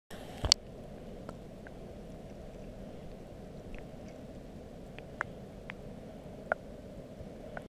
King Salmon Underwater
A quick view of some king salmon underwater.